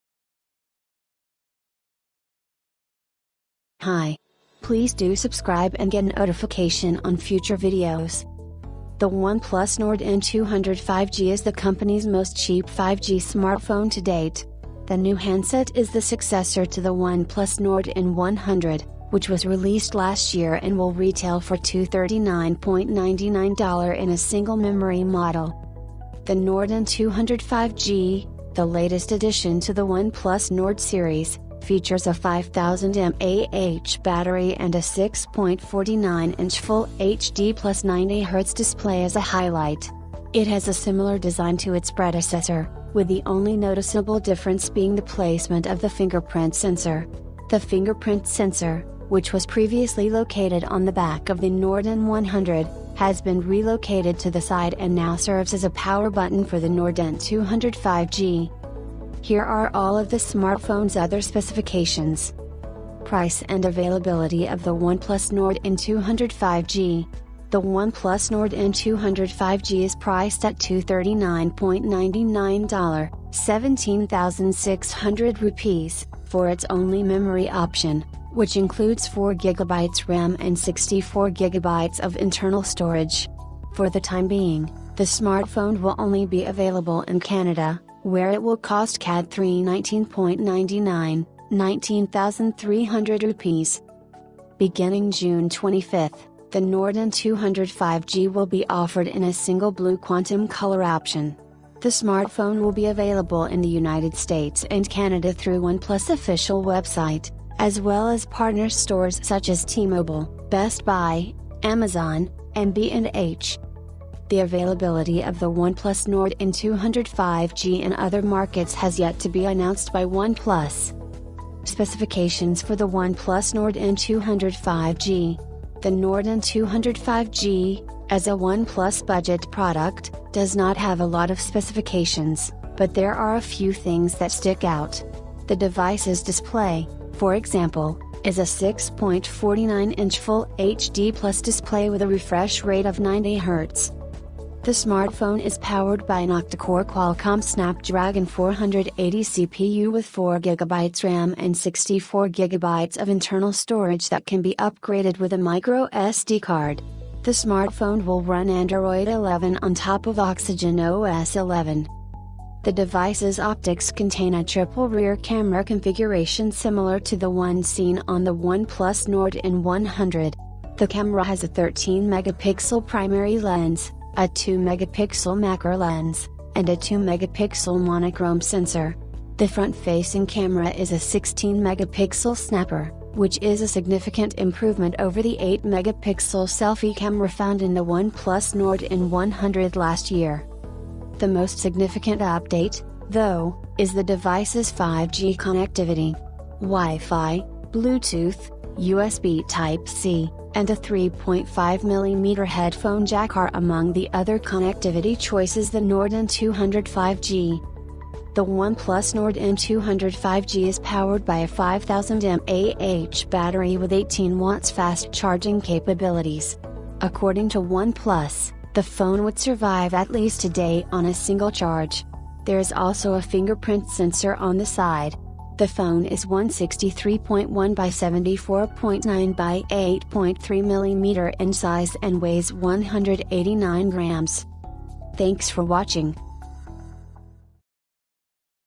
oneplus nord N200 - oneplus nord n200 - oneplus most affordable 5g-ready phone
oneplus nord N200 - oneplus nord n200 5g - is there any need of india launch!!!

Introducing the new oneplus nord n200 5g - which will be the mid - ranger device by oneplus with 5g connectivity and this will be the 5th nord device after oneplus nord  nord n10  nord 100  nord ce 5g and now oneplus nord n200 5g.